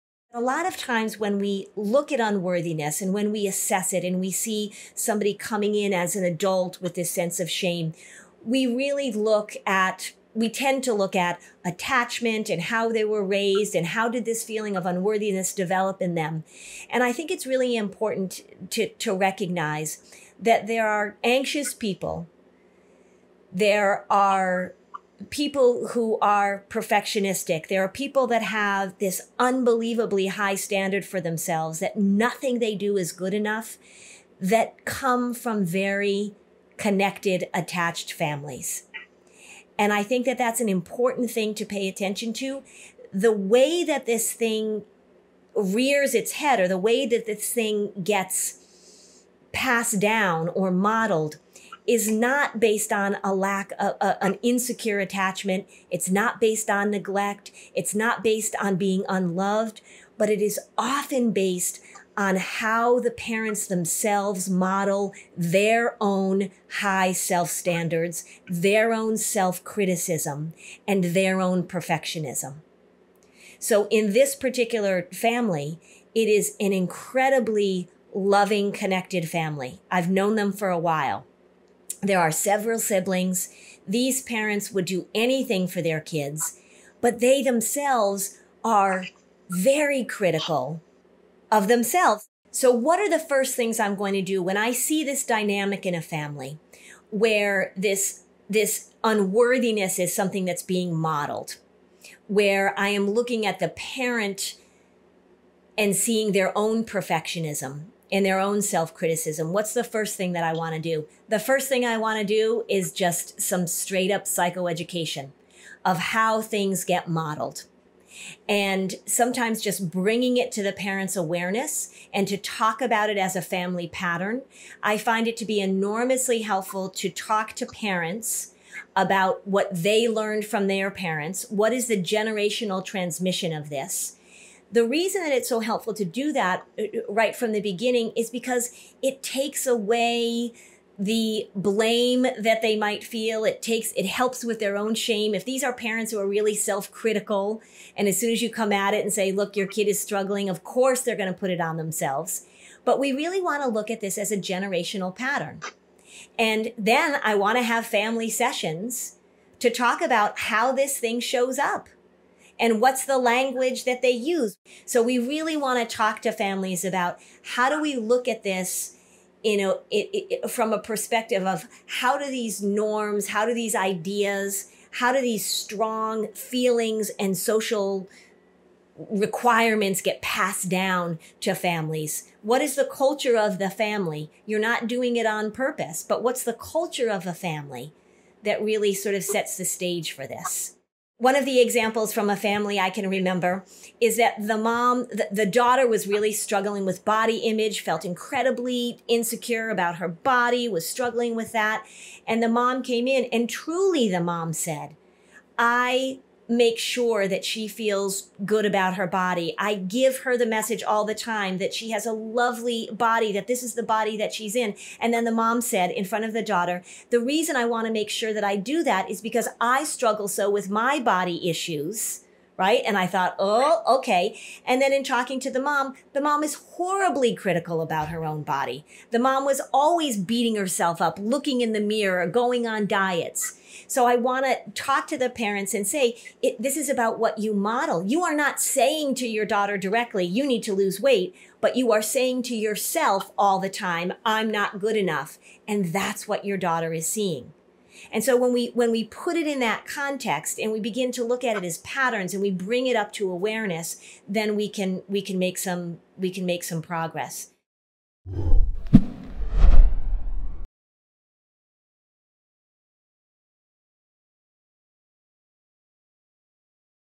How Feeling "Never Good Enough" Can Get Passed Down Through Generations
Many times, when a client reports feeling unworthy, they may be able to pinpoint specific instances when they received messages that they weren’t good enough.

But there are also indirect ways that this message can be conveyed and passed across generations – even in loving, connected families.

Lynn Lyons, LICSW, will tell you how.

NICABM - Better Outcomes. More Quickly.